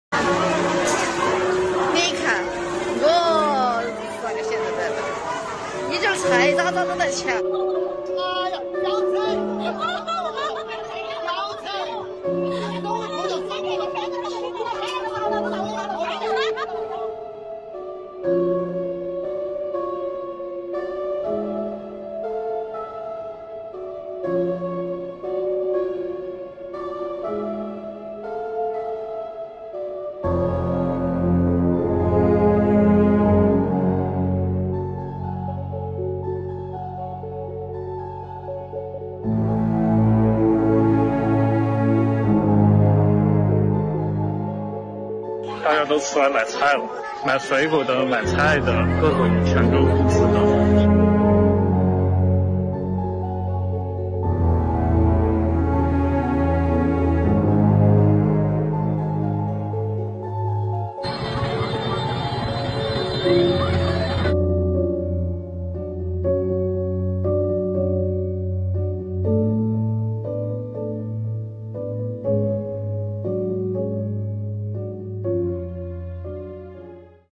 成都封城 市民又忙抢粮
继上海之后，中国又有一座超过2千万人口的城市按下暂停键。官方称是“加强社会面管控”，而未解读为“封城”或者“全域静态管理”。

因新冠疫情升温，成都周四（1日）傍晚6时起，全体居民原则居家，非必要不离蓉，形同“封城”。市民得知消息后，赶到超市抢购物资，超市里大排长龙，货架上的物资都被抢购一空。（画面取自网络）